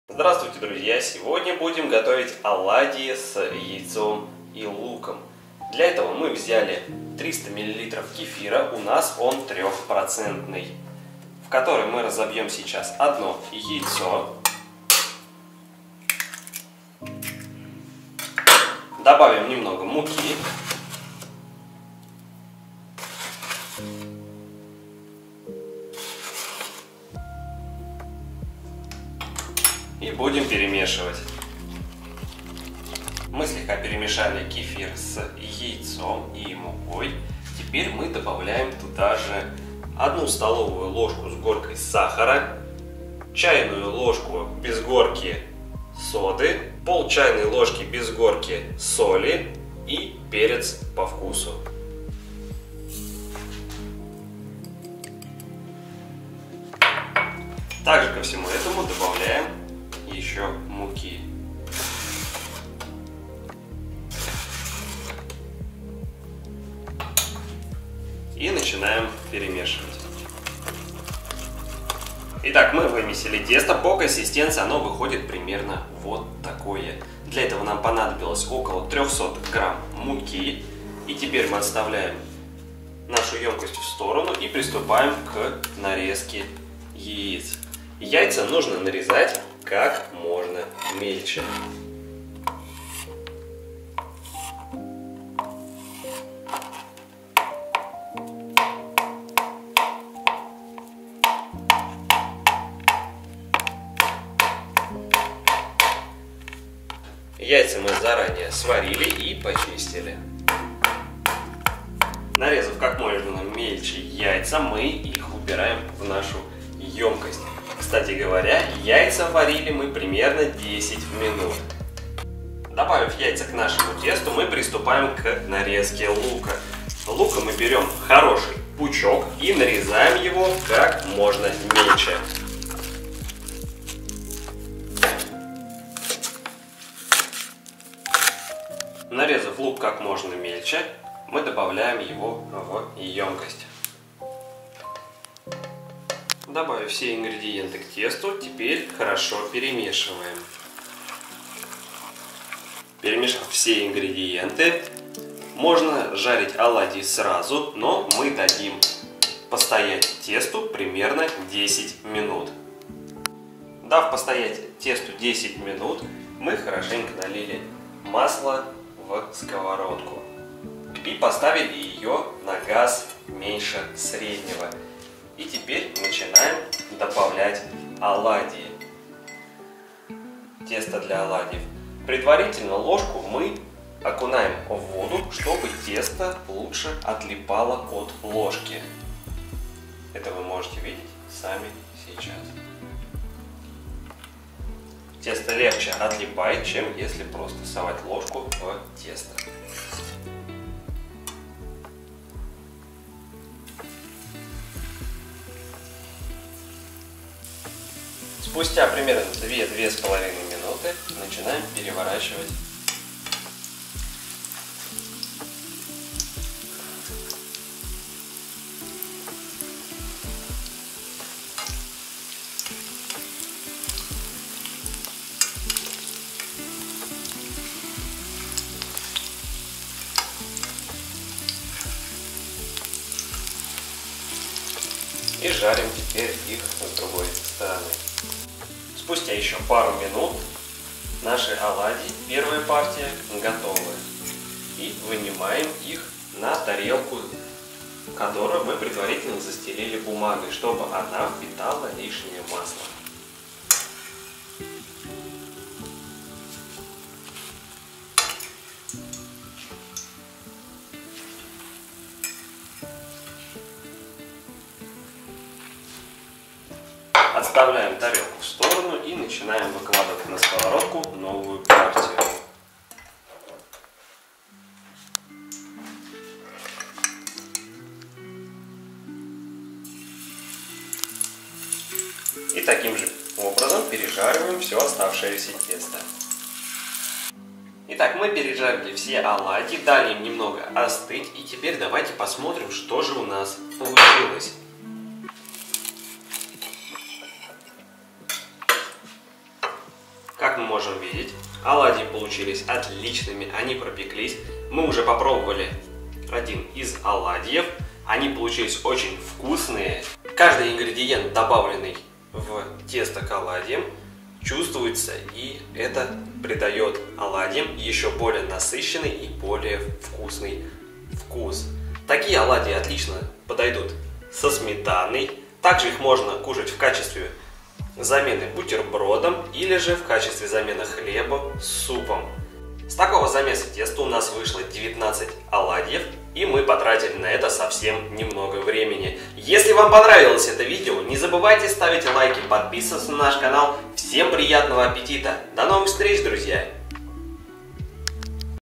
Оладьи На Все Случаи Жизни
Этот рецепт оладьев пользуется большой популярностью у домохозяек и мам, что готовят их своим домашним к обеду или ужину
Вот и мы решили поделиться с вами этим рецептом
Присаживайтесь по удобнее, мы начинаем👌